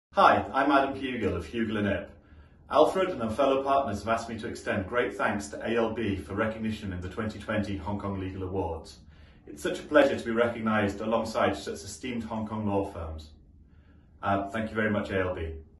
Hugill & Ip awarded Dispute Resolution Boutique Law Firm of the Year by Asian Legal Business
Asian Legal Business (ALB) presented the 19th Annual ALB Hong Kong Law Awards 2020 on 27 November – the pre-eminent legal awards and ALB’s biggest and longest-running awards in Asia.

Hugill & Ip has been awarded the winning prize for Dispute Resolution Boutique Law Firm of the Year and has been shortlisted as finalist in 8 other categories:
- HKGALA Award for Corporate Citizenship
- Immigration
- Labour and Employment
- Matrimonial and Family
- Private Wealth
- Rising Law Firm
- Tax and Trusts
- Transactional Boutique Law Firm